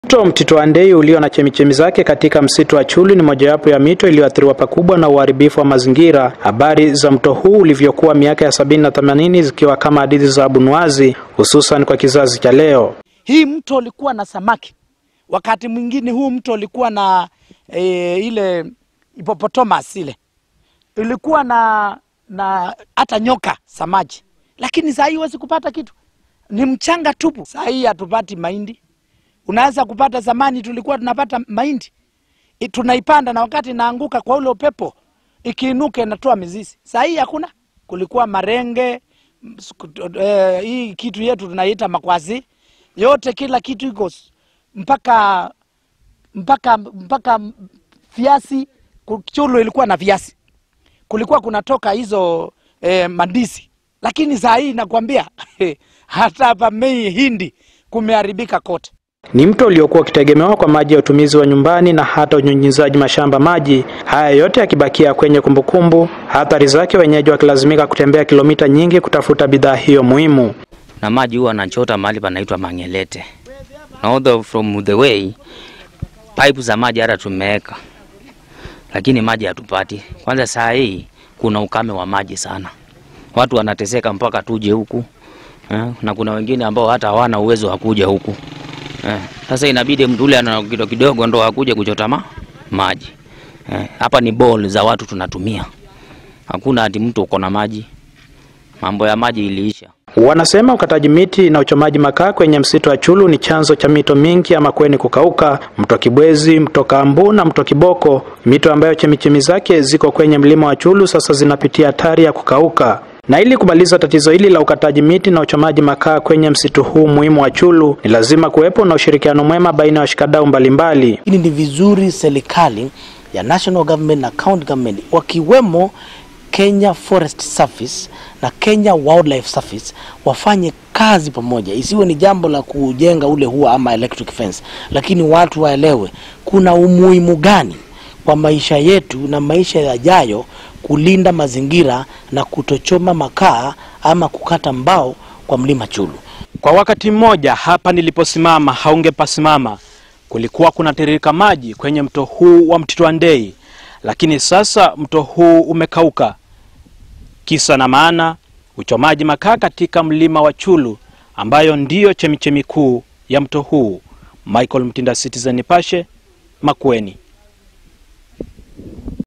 Chemchemi za maji zakauka katika msitu wa Chyulu
Ukataji miti na uchomaji makaa katika msitu wa Chyulu kaunti ya Makueni unatishia kuangamiza chemichemi zote za maji. Tayari mito sita iliyokuwa ikitengemewa kwenye kaunti hiyo imekauka kutokana na uharibifu wa mazingira.